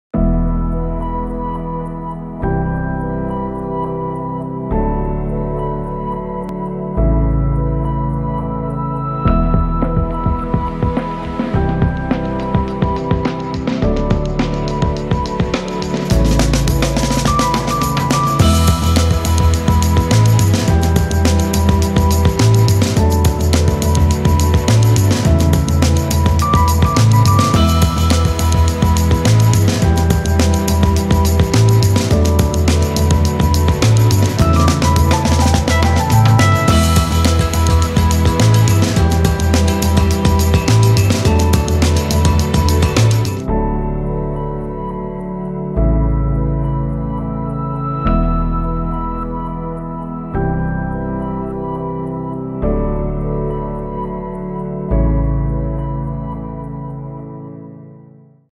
Create a reliable and sound surface with Palace Self-Level
You can rely on Palace Self-Level to create a smooth and consistent finish prior to laying ceramic or decorative floor coverings onto internal concrete floors. Mix with Self-Level Latex when working on floors where under tile heating is to be specified or a more flexible finish is required, like timber substrates.